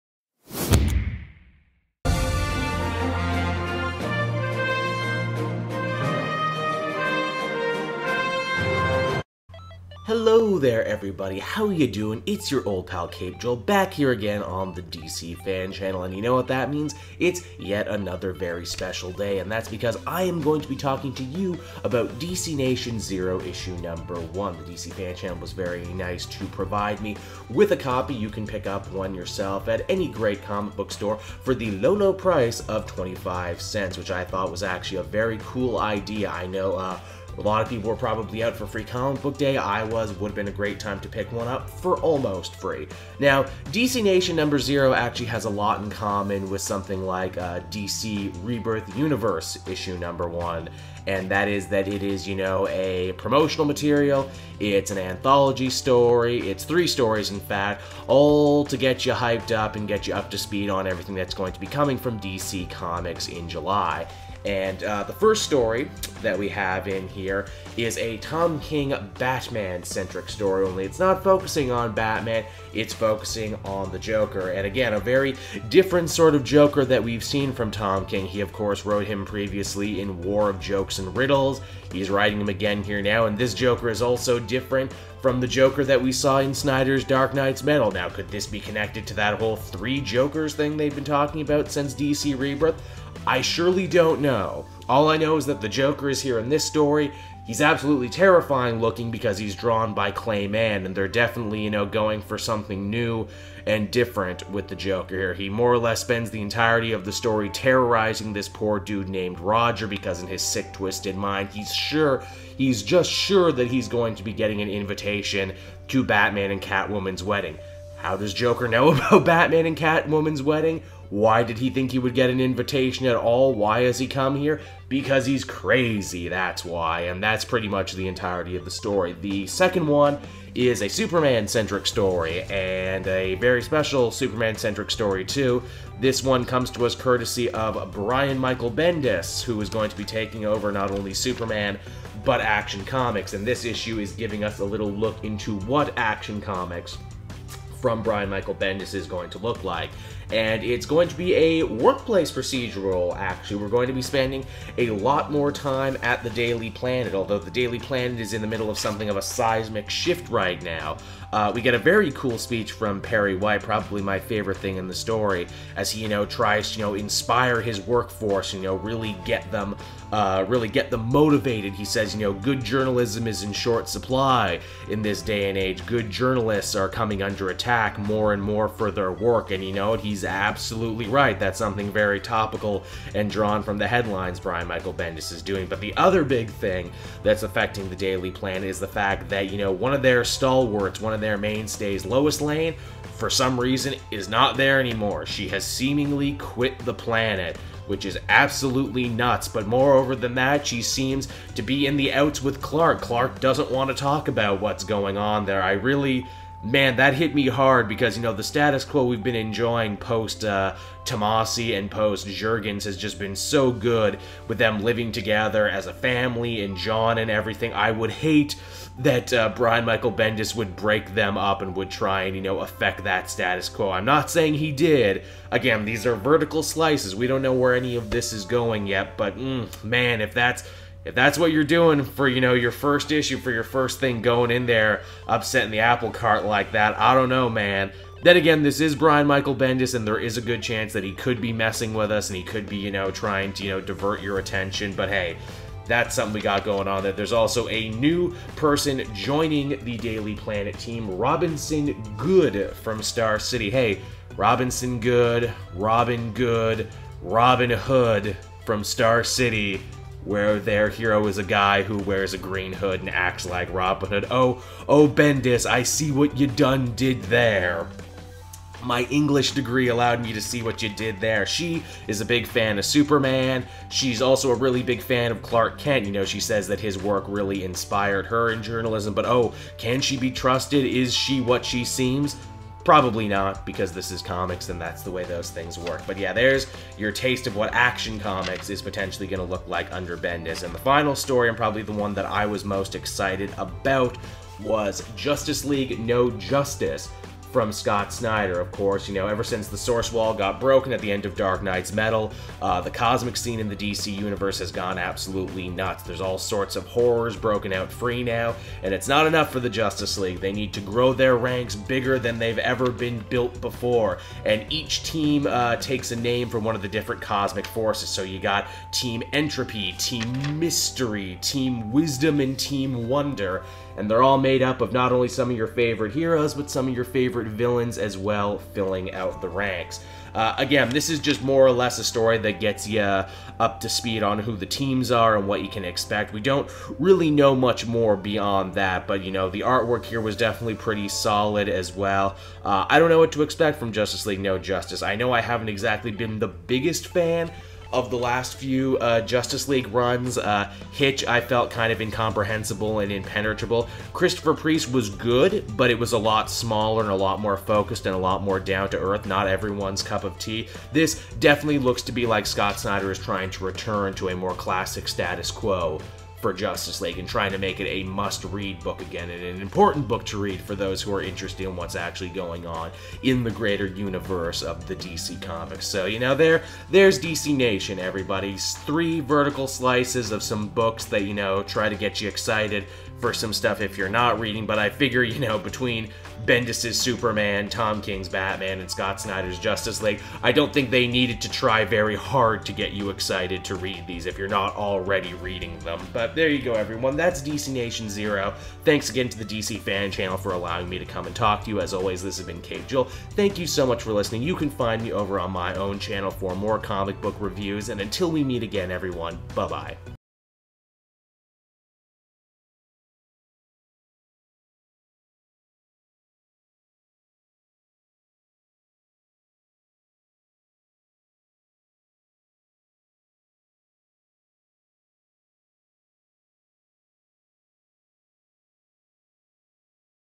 DC NATION #0 Review By Caped Joel
Celebrate the recent release of DC NATION #0 with Caped Joel and see what more is to come in terms of new story lines and more.

Let us know what series you are excited to follow in the comments below.